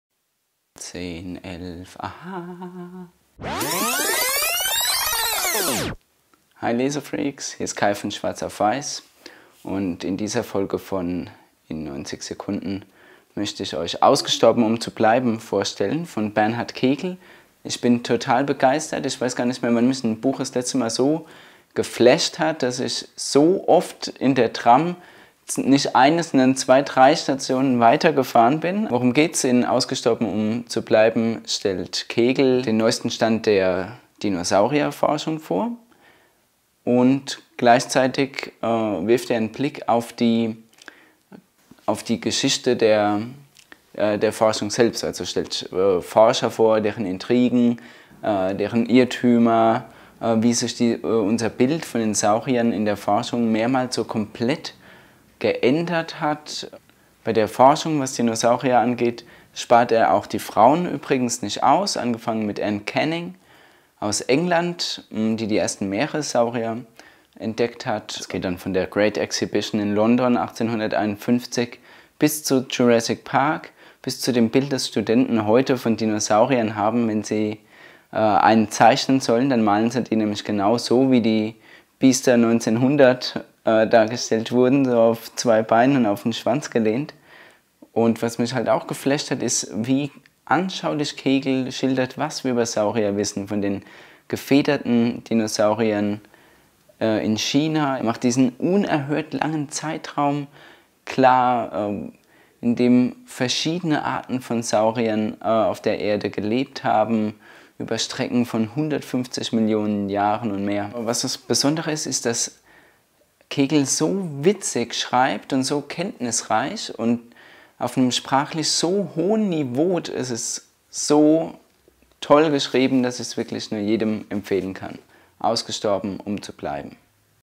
Ausgestorben um zu bleiben | 90 Sekunden | booktube | schwarzaufweiss
Klare Empfehlung für: Bernhard Kegel: Ausgestorben um zu bleiben. DuMont, 260 S.,
Freue mich auf eure Kommentare!